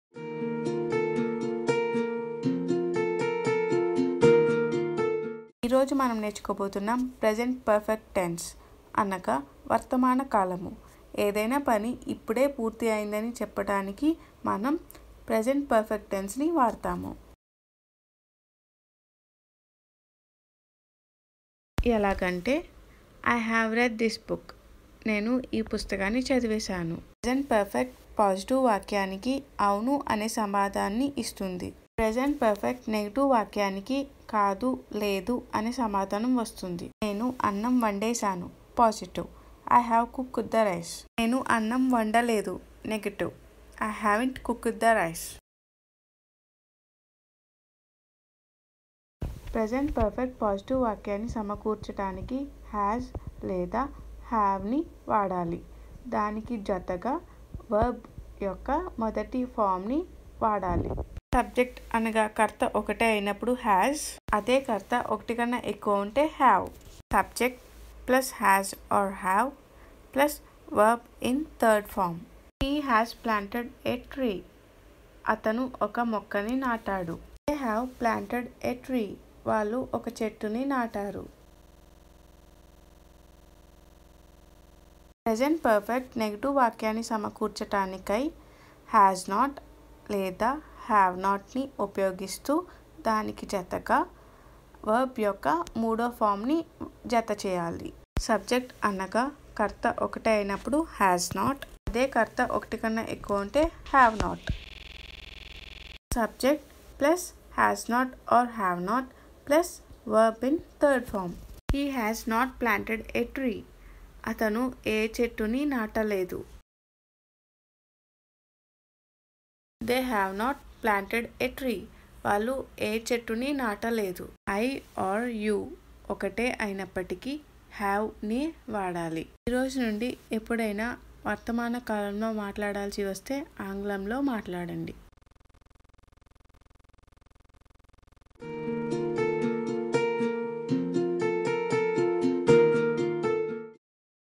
Basic English Grammar in Telugu | Present Perfect Tense | తెలుగులో ఆంగ్ల వ్యాకరణ
English Grammar Lessons for Beginners, Kids and Children in Telugu. Watch the grammar videos, these grammar videos help you learn and speak correct English.
To learn English from Basics download our free app on the Playstore

How to learn English? How to learn English grammar?